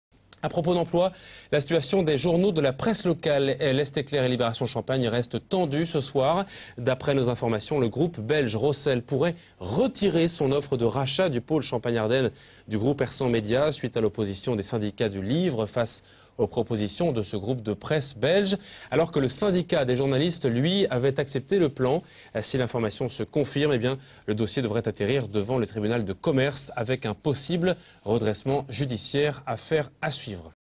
Situation tendue chez L'Est Éclair et Libération Champagne
Sites webs/blogs, copiez cette vidéo avec le bouton "Intégrer" !
 Le groupe belge Rossel pourrai retirer son  offre de rachat du pôle Champagne Ardenne du groupe Hersant Media